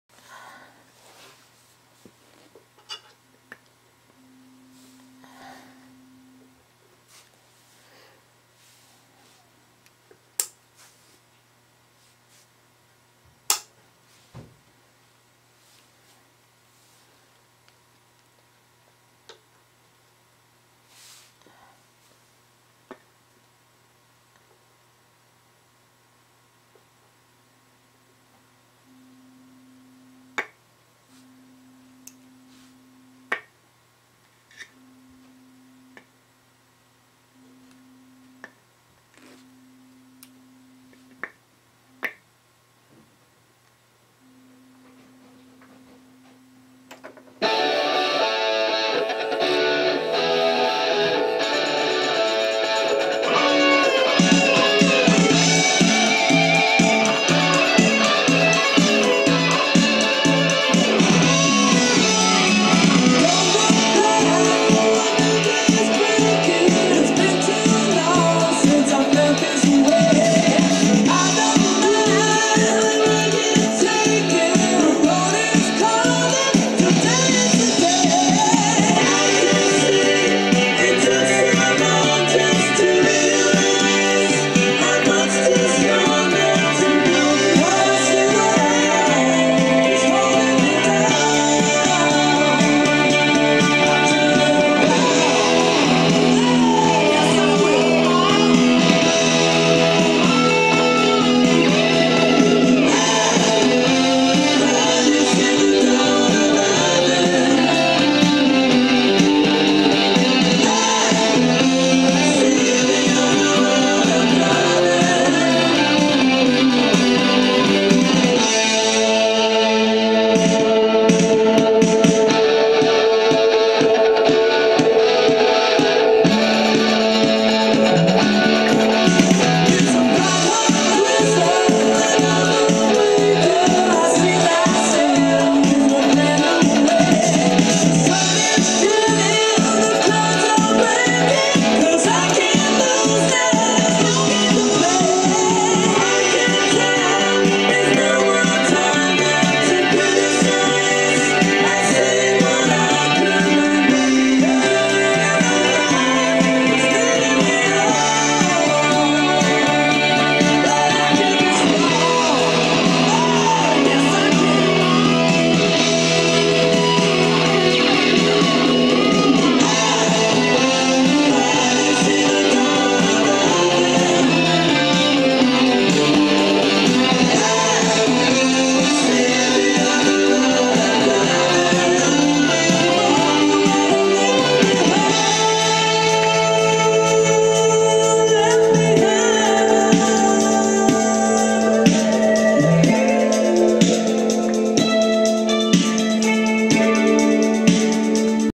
Stethoscope Acoustic Transmission Test Fixture
This is a clip of a Stethoscope Acoustic Transmission Test Fixture we designed, to test rudimentary functionality of a stethoscope - chestpiece working, all tubing clear. The design challenge was that it had to work in a noisy manufacturing environment, perhaps not quite this loud, but you get the idea. The audio test signal is very low in amplitude, barely perceptible, to simulate realistic use conditions. Observe that the test fixture performance is unaffected by the ambient noise.